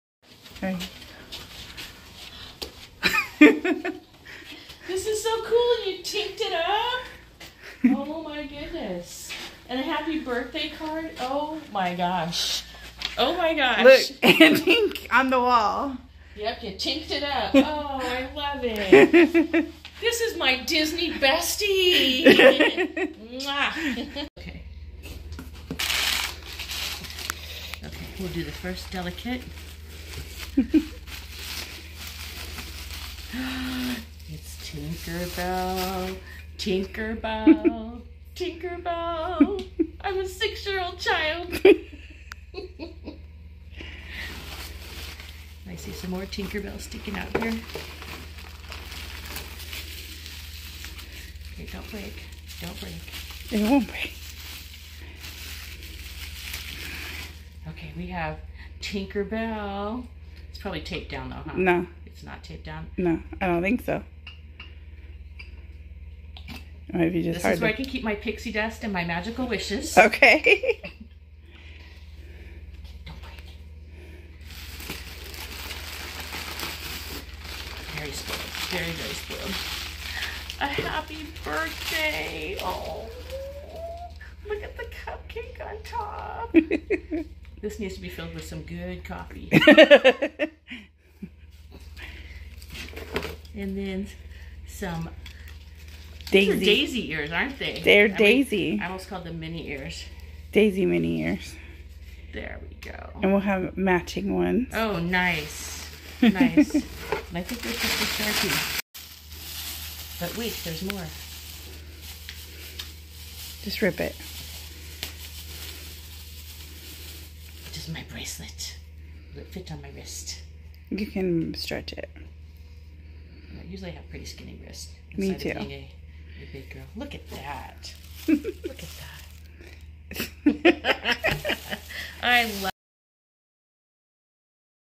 BIRTHDAY SURPRISE FOR BESTIE BIRTHDAY!
best friend DISNEY Birthday

She had never been celebrated like this before ! it was an amazing way to start that week ! best vacation we both had in a while !
we ate we went shopping !
went to Disneyland . we killed it